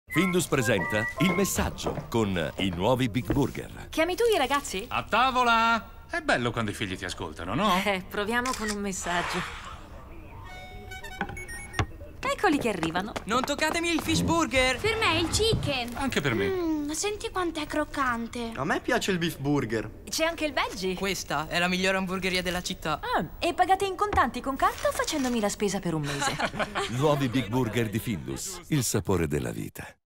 I Big Burger Findus in tv con Havas Worldwide Milano
DescrizioneUn’accogliente casa diventa una gustosa “hamburgeria” nel nuovo soggetto della campagna Findus dedicata a “Il sapore della vita”, on air dal 30 novembre.

Questa volta, una mamma e un papà sono dietro ai fornelli, intenti a preparare i Big Burger Findus per accontentare i gusti di tutti... figli compresi!

Grazie a uno scatto fotografico deiburger che la mamma invia ai tre giovani figli, in pochi secondi la tavola si popola.

Ce n’è per tutti i gusti e per tutti i palati: dal Veggie Burger, il vegetariano, al Fish Burger, ricco di filetti di Merluzzo d’Alaska; dal Beef Burger, preparato con carne bovina selezionata, al Chicken Burger, il burger di pollo avvolto in un croccante guscio di riso soffiato.

Con la complicità dei Big Burger, la simpatica famiglia si ritrova così a tavola a chiacchierare e a condividere momenti pieni di affetto e giovialità.

La creatività dello spot tv è curata dall'agenzia pubblicitaria Havas Worldwide Milano.

Credits:

Direttore Creativo Esecutivo: Giovanni Porro
Direttore Creativo (copy): Luigi Fattore
Direttore Creativo (art): Antonio Campolo
Account Director: Manfredi Calabrò